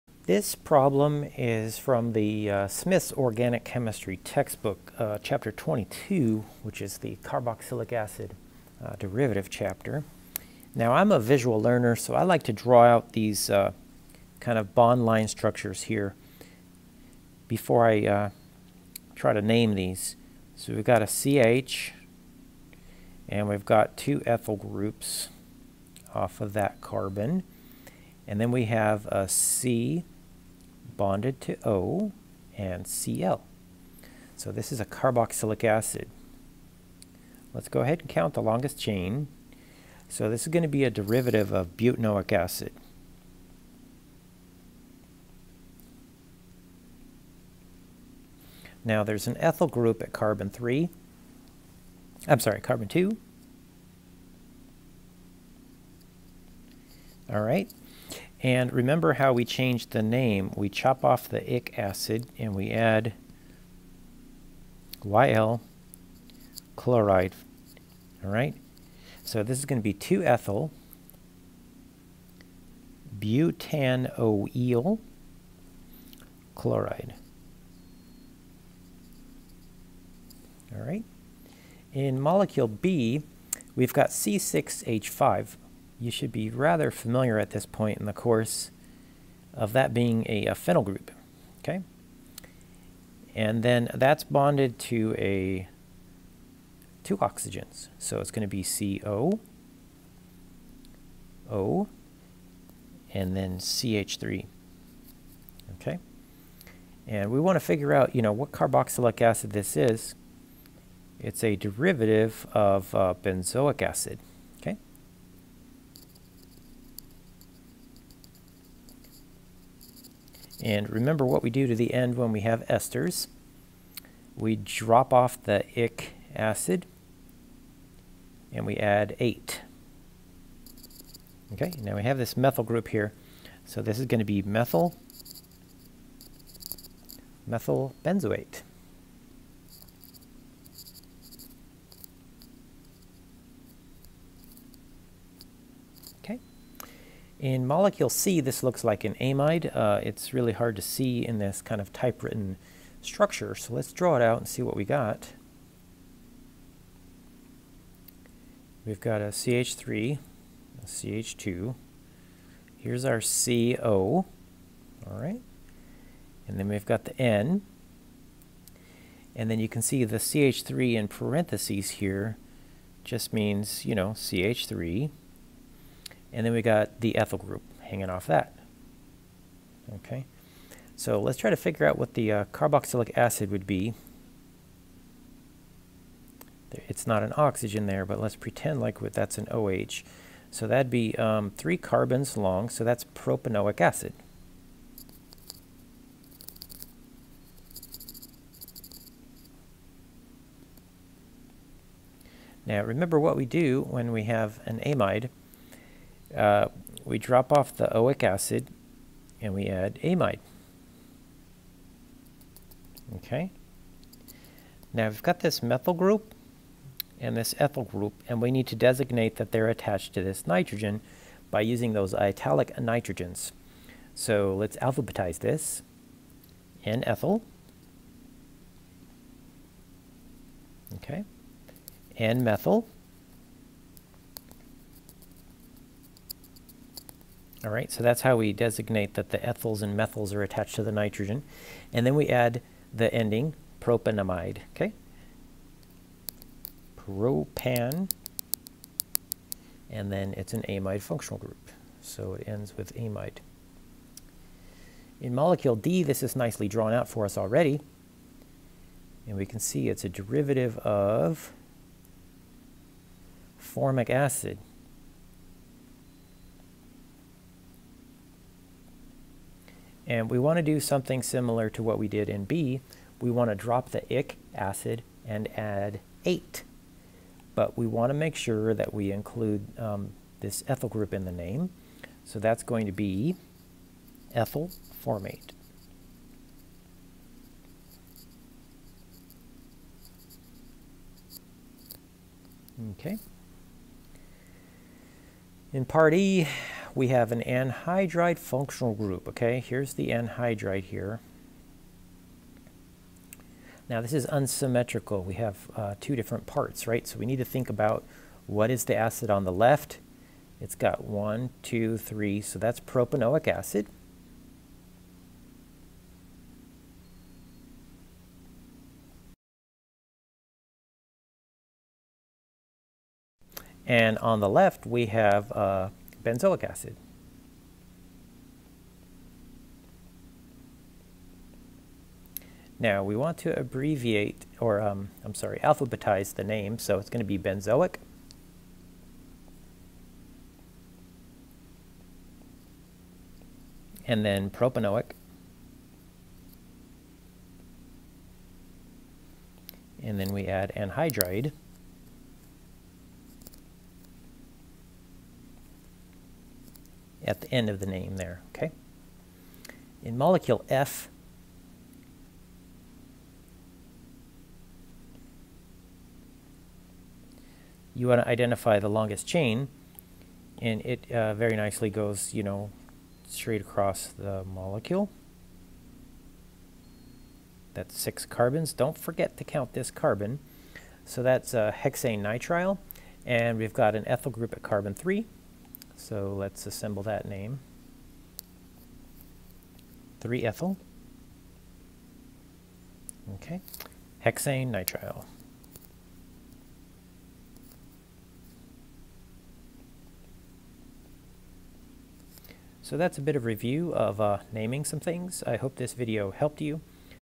Naming Carboxylic Acid Derivatives [ORGANIC CHEMISTRY] Smith 22.4
This problem comes from the Smith Organic Chemistry textbook. Chapter 2, problem 4